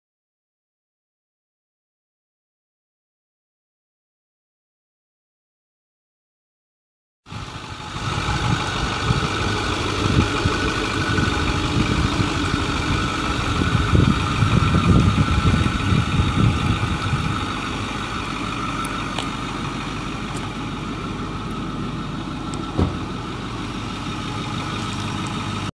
2008 Ford F250 Super Duty XL pickup truck for sale | no-reserve Internet auction March 24, 2016
2008 Ford F250 Super Duty XL pickup truck
215,143 miles on odometer
6.4L V8 OHV 32V turbo diesel engine
Automatic transmission
AC and heat
Tilt
Cruise
AM/FM
Manual windows and locks
Vinyl interior
Fuel tank
Pump
Cross bed toolbox
Long bed
245/75R17 tires
Tail gate latch is difficult to operate
1FTSF20R48ED67130

It's located in Norton, KS, and we're selling it to the highest bidder regardless of price.